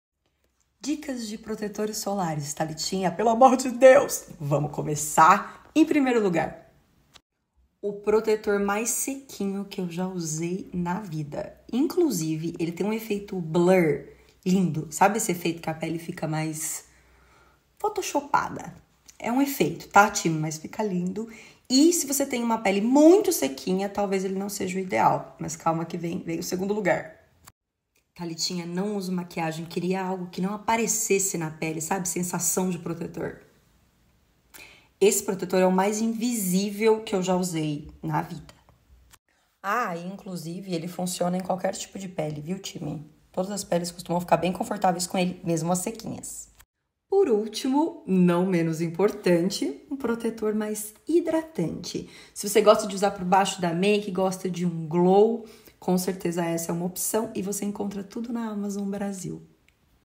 Protetor solar p/ todos os tipos de pele
Vocês sempre me pedem sugestões de protetores solares, time 🥰

* Produtos mencionados no vídeo:

1. Protetor solar Neostrata - Toque Seco e oil control

2. Photoage Water Dermage - Toque seco e oil free

3. Bioré Aqua Rich

♥️ Publicidade da Talitinha!